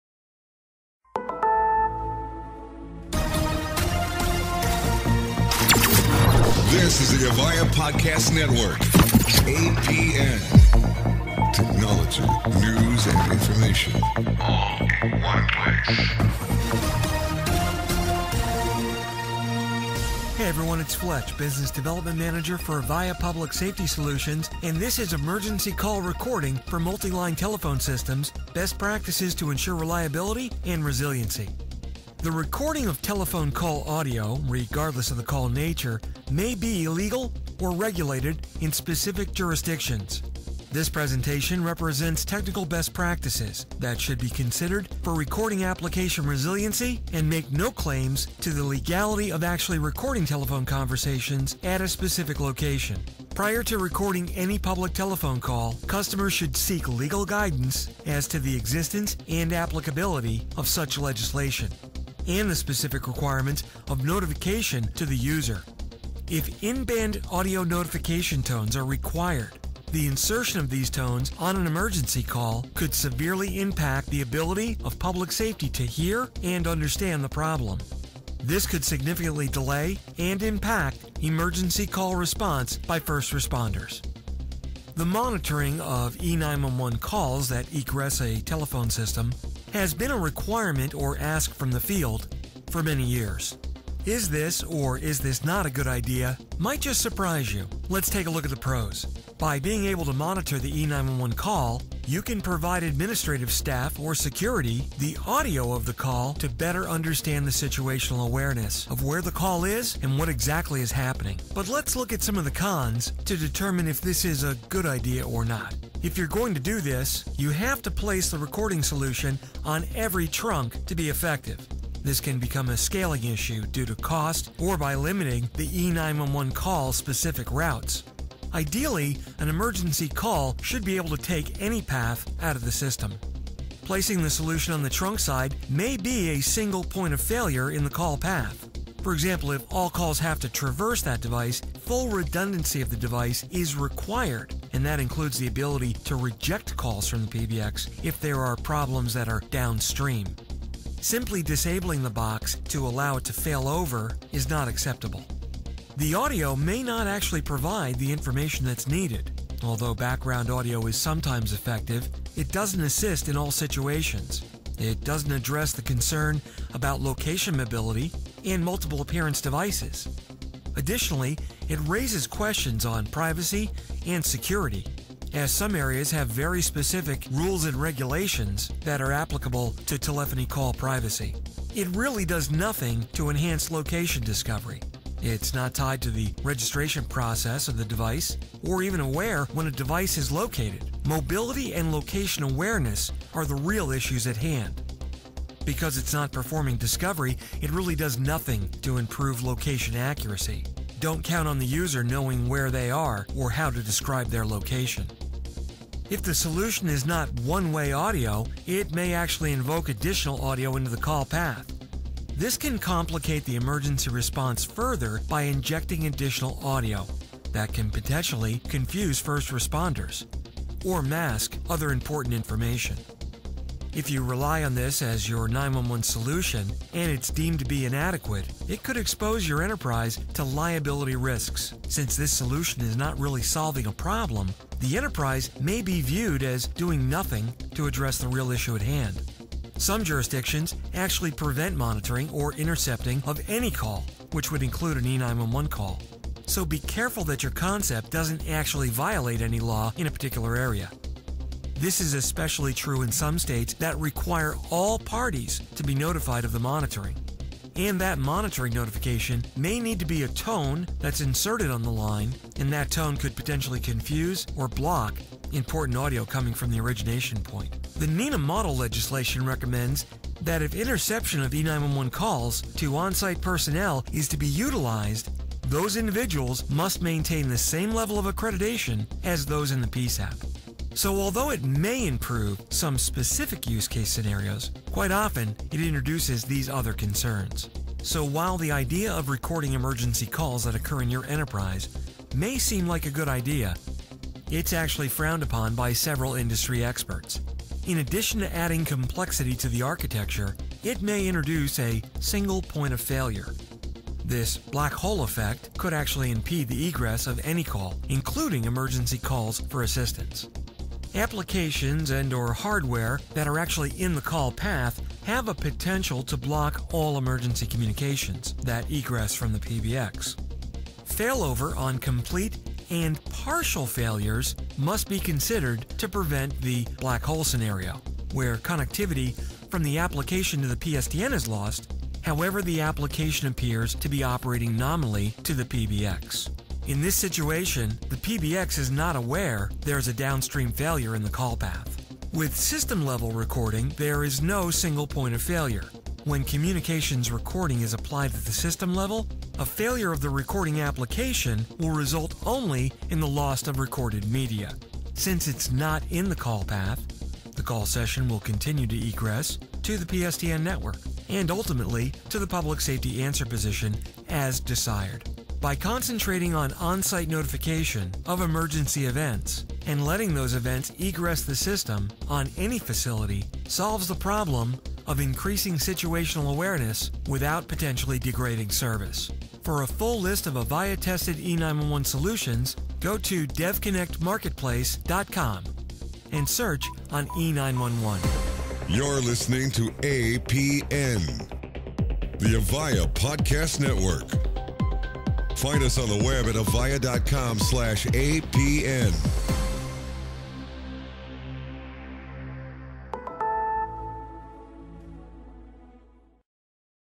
MLTS E911 Recording Best Practices
Is recording your E911 calls a good idea? Is it even legal? Understand how to minimize your liability by understanding these basic facts around the good and bad of any type of call recording, especially E911 calls.